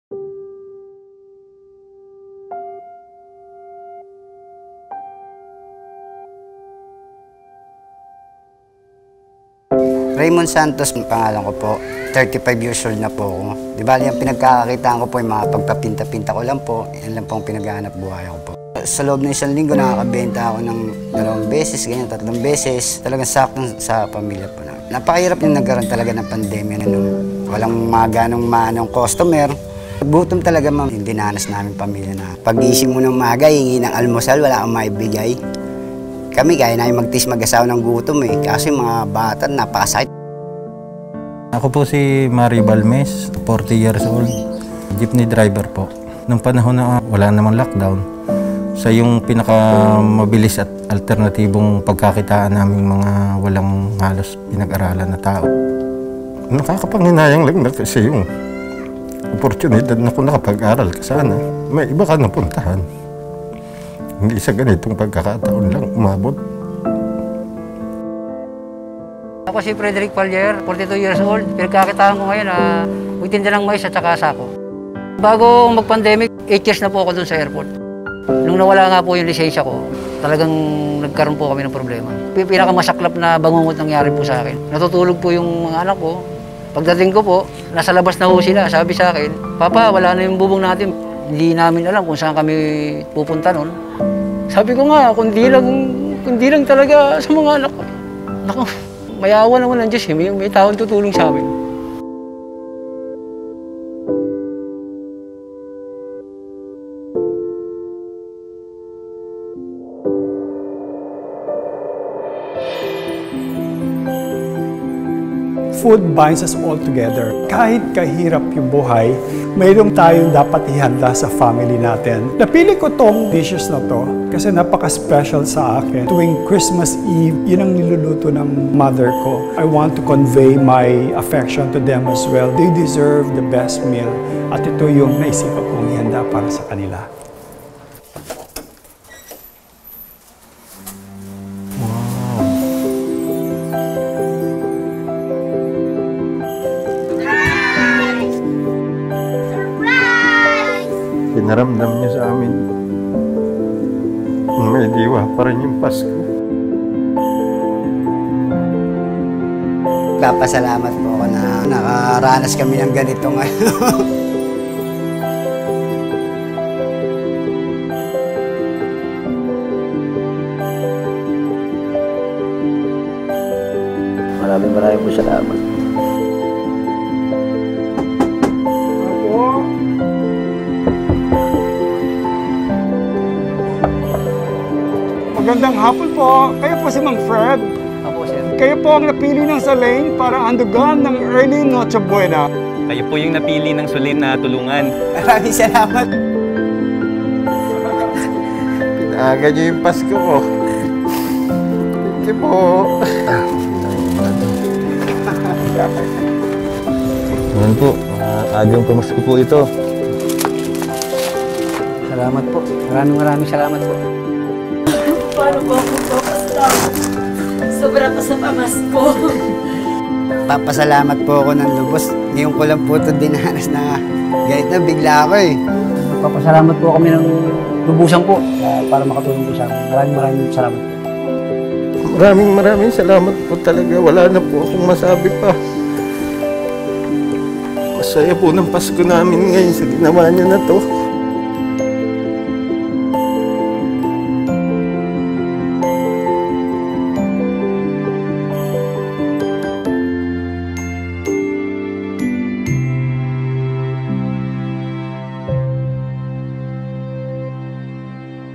Alab ng Pasko
Sinubok man tayo ng pandemya, patuloy pa rin tayong lumalaban. Lumalamig ang bawat araw ngayong parating na ang Pasko, ngunit may dalang alab ng pag-asa ang taos-pusong pagmamalasakit at yakap ng ating pamilya.